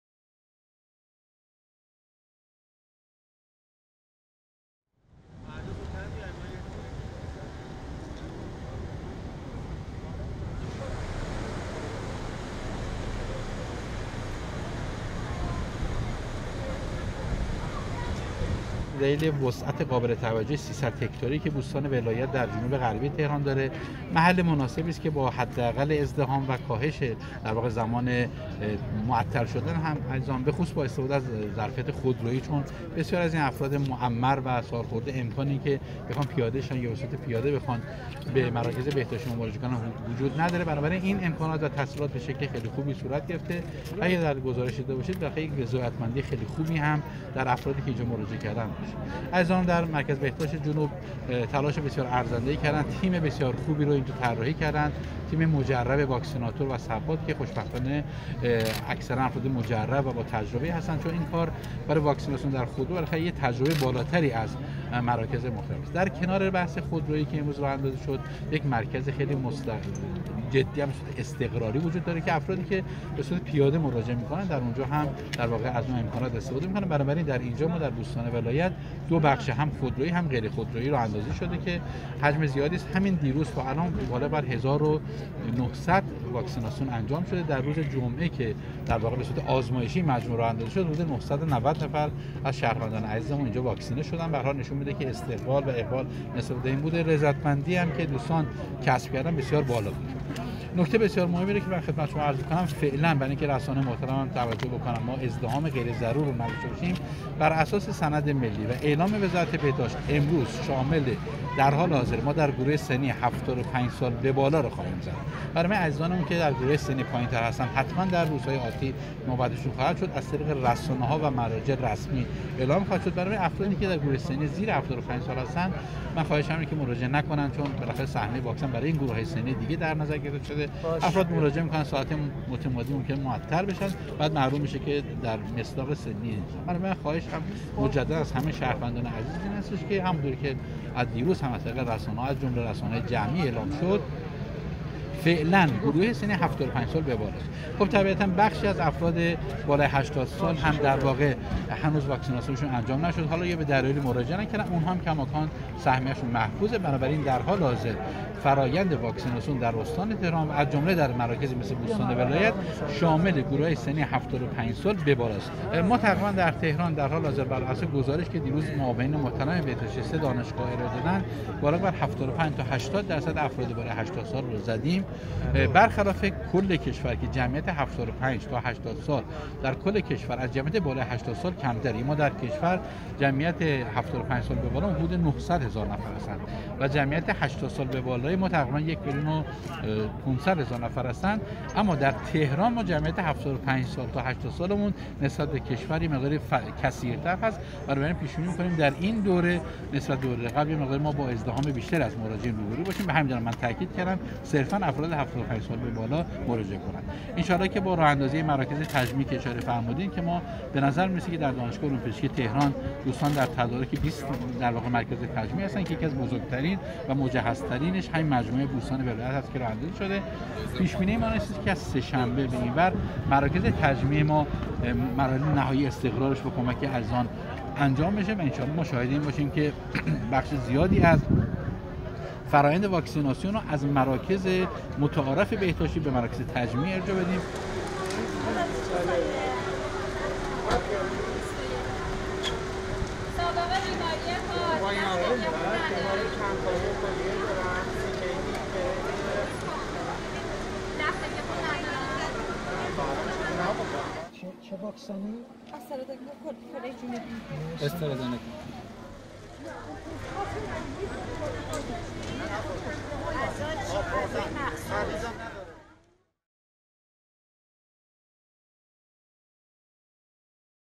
افتتاح بزرگترین سایت خودرویی تزریق واکسن در تهران
بزرگ‌ترین سایت خودرویی تزریق واکسن کشور صبح یکشنبه (۲۶ اردیبهشت) در بوستان ولایت تهران افتتاح شد. به گفته مسئولان ستاد کروناف فاز اول واکسیناسیون کووید-۱۹ شامل کادر بهداشت و درمان به پایان رسید و واکسیناسیون افراد بالای ۸۰ سال نیز رو به پایان است و از روز گذشته واکسیناسیون بالای ۷۵ سال آغاز شده است.